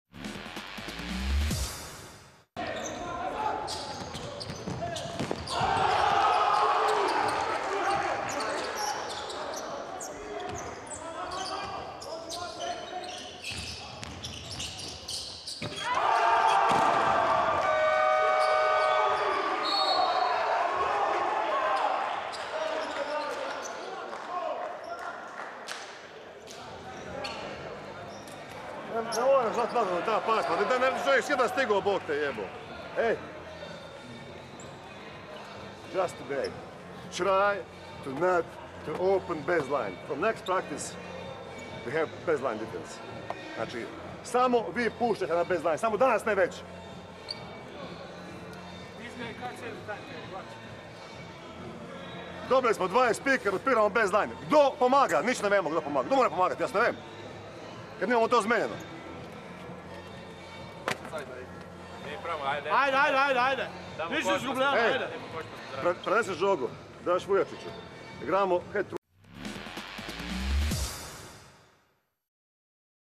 This Mateo Čolak's sequence is just SPECTACULAR! (Rogaška - Široki, 11.11.2021)
WOW! You just have to see this two plays made by Široki player Mateo Čolak!

Čolak firstly made a MONSTER block, while just couple of moments later he threw down a two-handed dunk to put his team in front by 5 points.